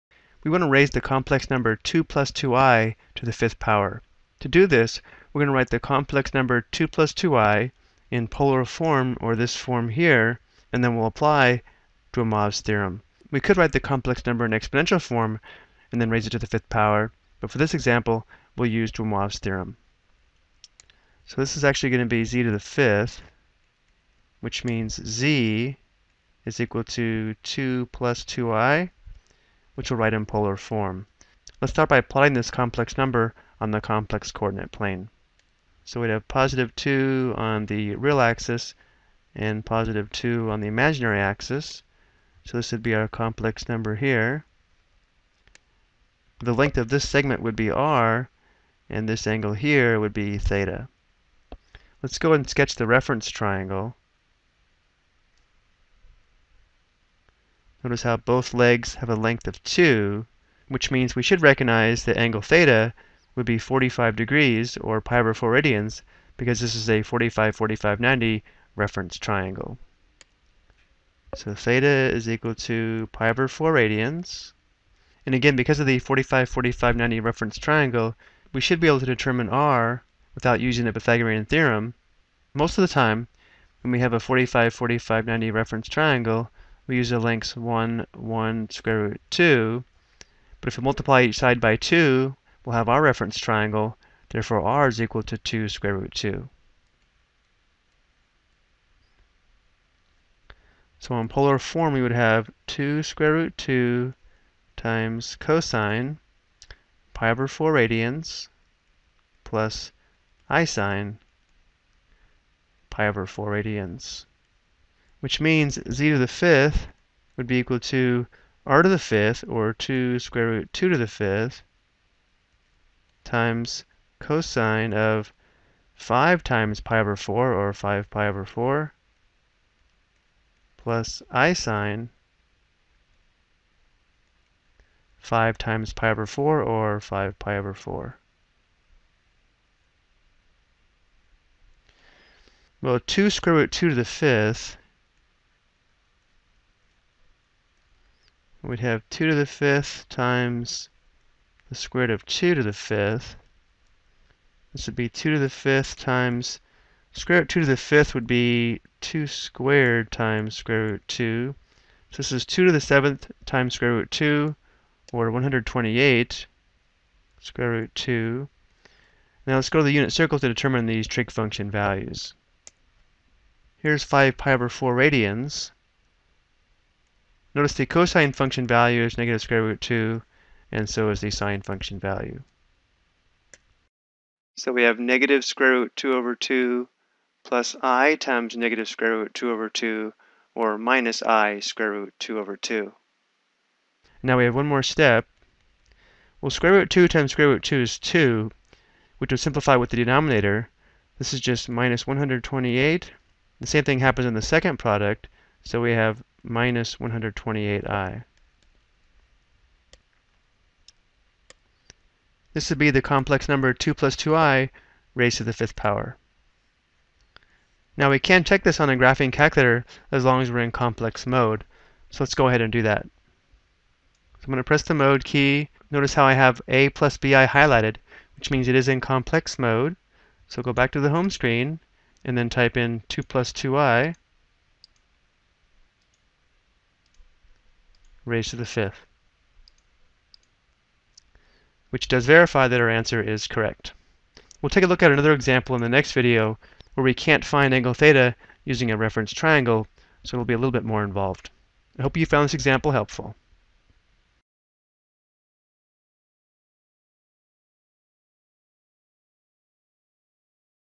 Ex 1: Raise a Complex Number in Cartesian Form to a Power - Demoivre's Theorem
This video explains how to use Demoivre's Theorem to raise a complex number in Cartesian form to a power.  The angle is a common reference angle.